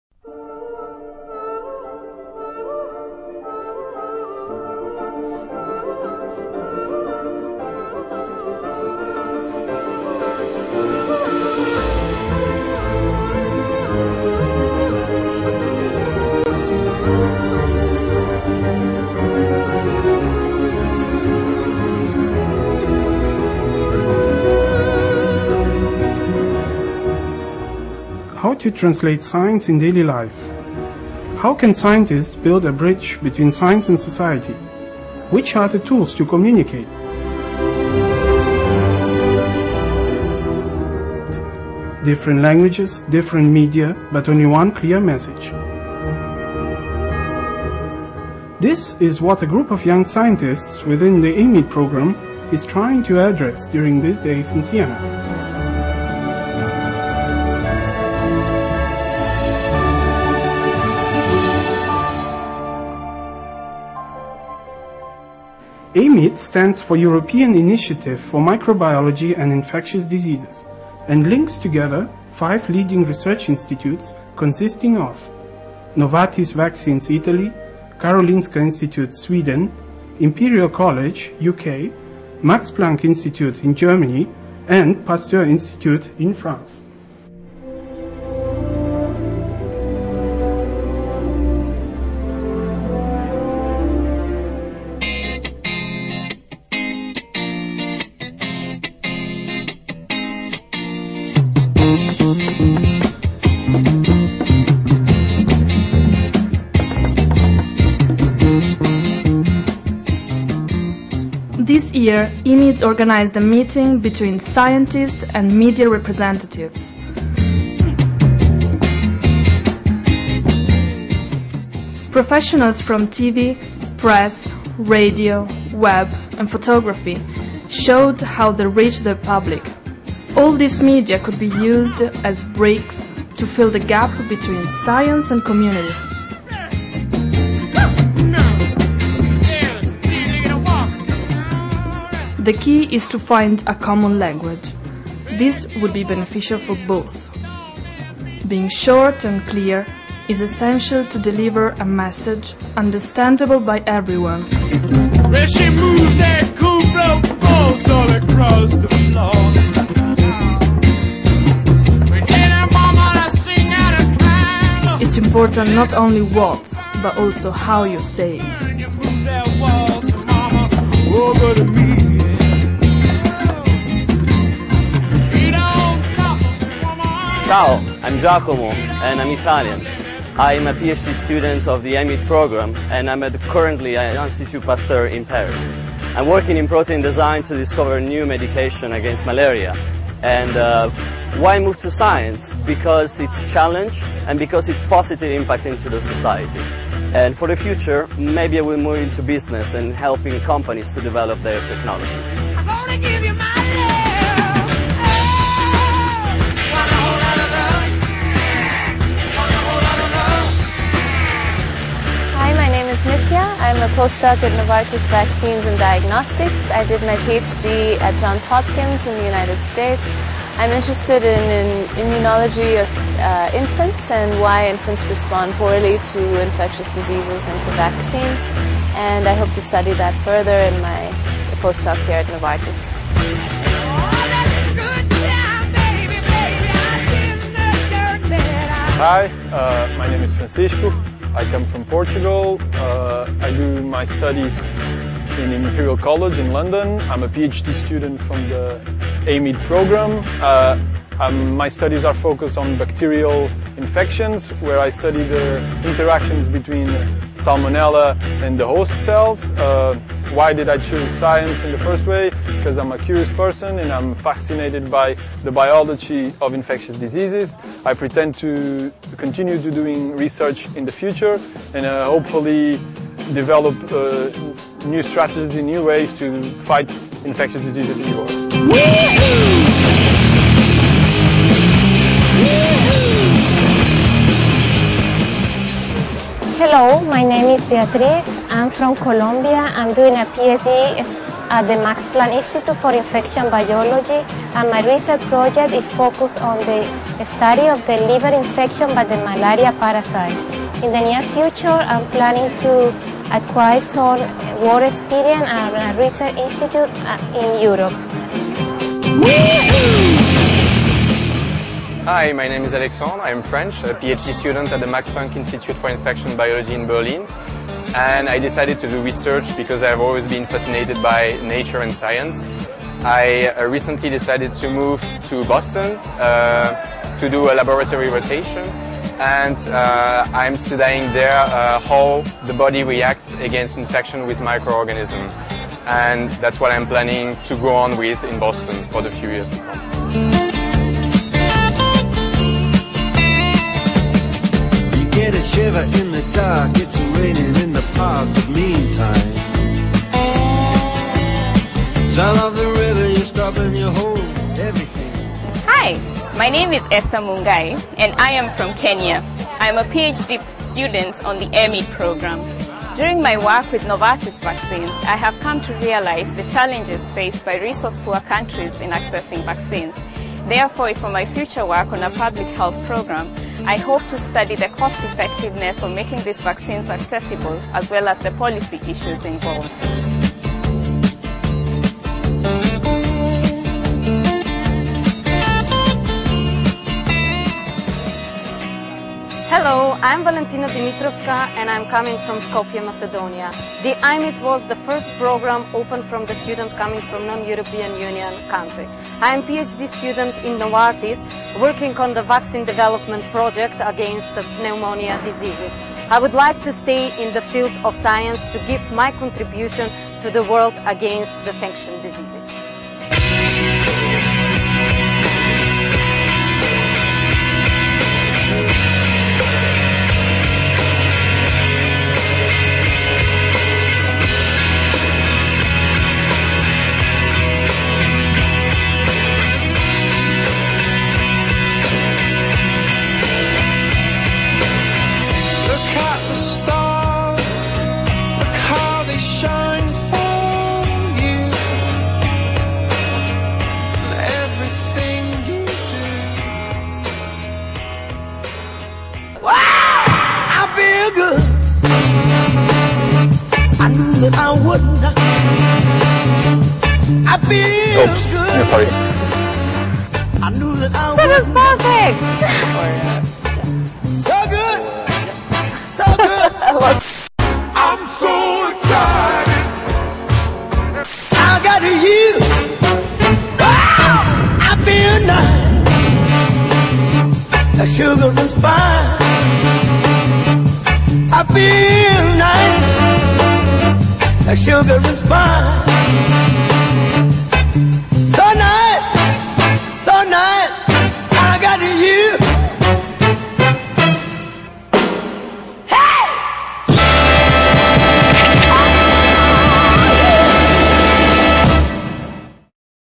EIMID - Jumping into science
This video has been generated during a course for young scientists that has been held in Siena in April 2009: "Scientific communication to general public: different languages, different media, but only one clear message".
The course and this movie were sponsored by EIMID-EST a training project financed by the Marie Curie Actions of the European Commission and coordinated by ALTA, a service-provider company specialized in developing and managing scientific research and training projects. This video was recorded in Siena (Italy) with the contribution of Canale 3 TV studios.